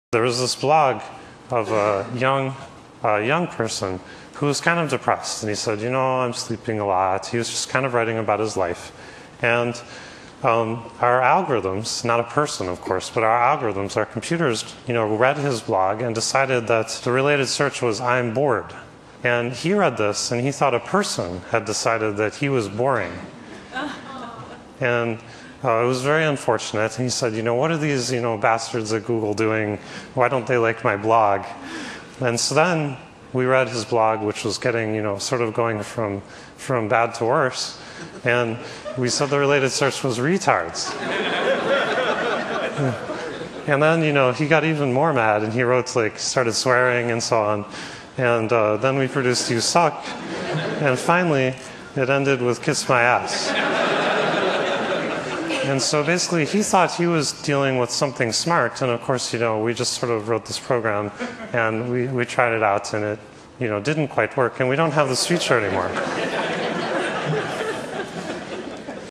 Inside the Google Machine- Anecdote: Blog and Algorithms
Thanks to TED. A story about a blog that didn't turn out so well. Google's algorithms weren't smart enough and a person's ramblings went "from bad to worse".